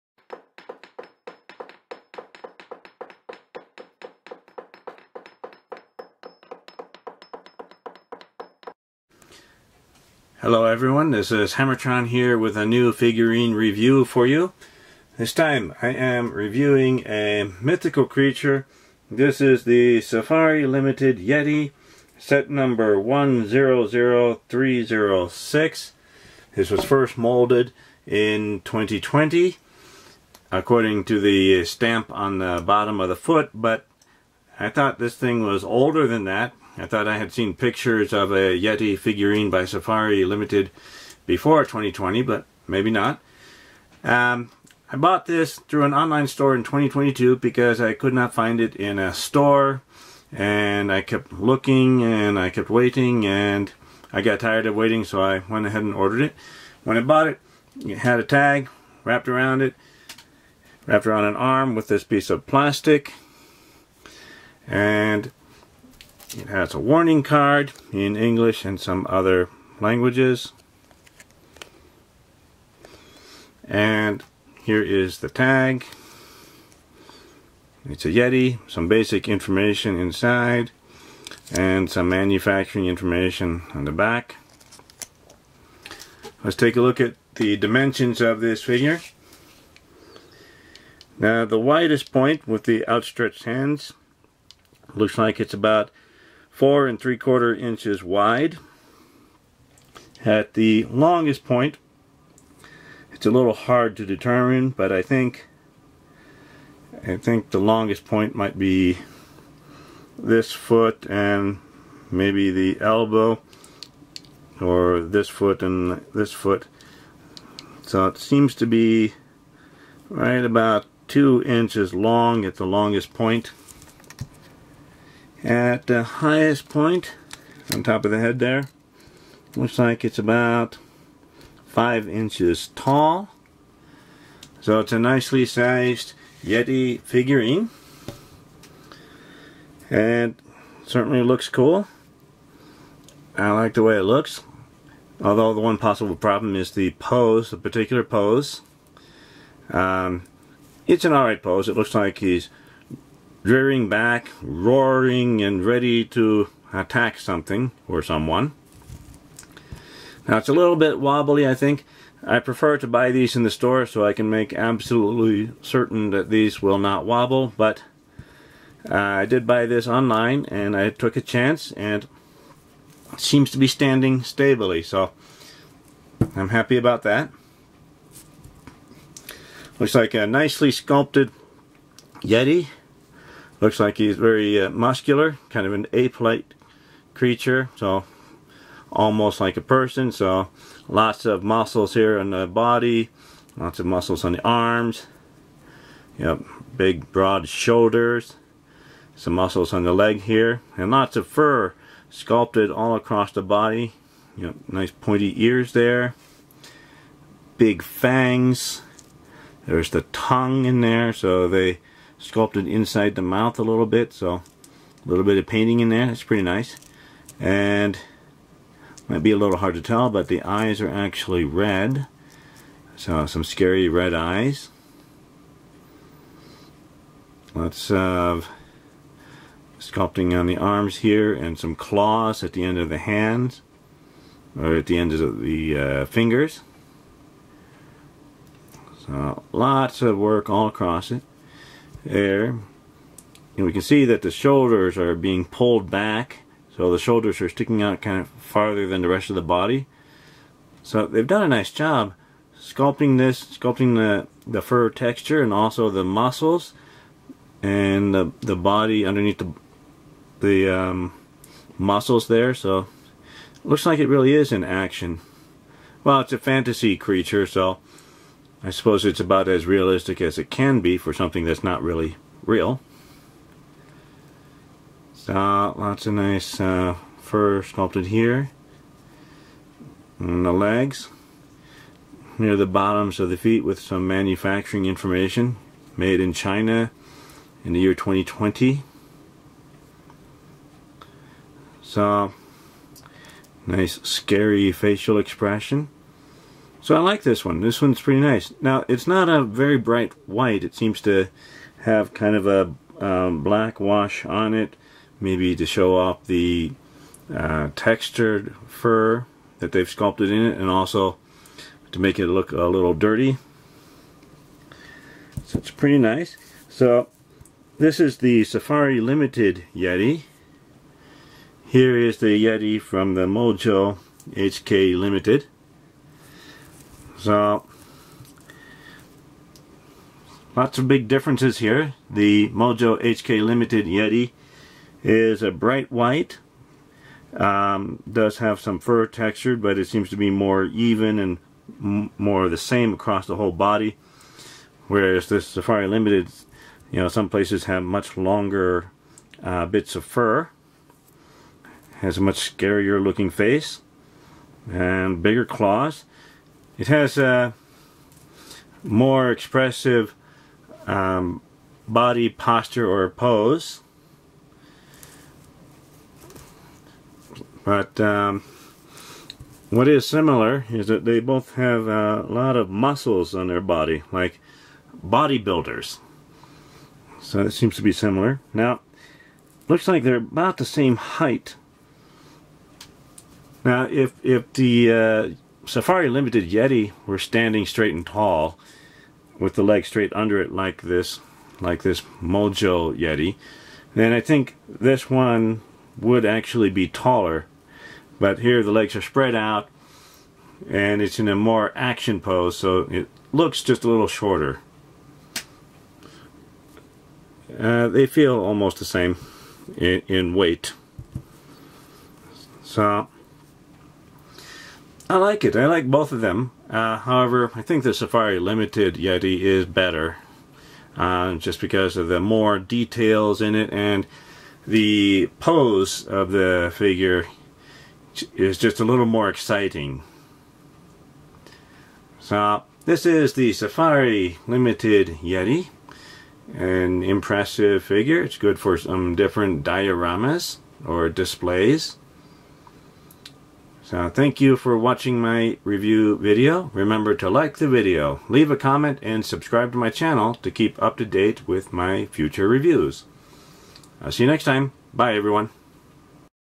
Safari Ltd. Yeti set # 100306 review
This is a review of the Safari Ltd. Yeti set # 100306.

This figure was first molded in 2020.

I bought this figure through an online store in 2022.